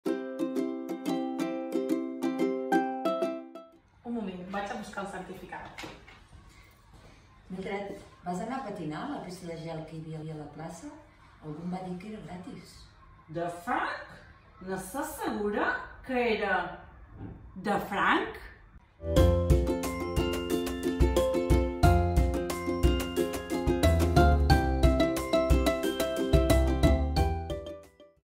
Desvetllamots 11: "de franc"
Ens ajudes a recuperar mots que estan caient en desús?
Proposem desvetllar l’expressió "de franc" (en comptes de "gratis", tot i que també és adequada).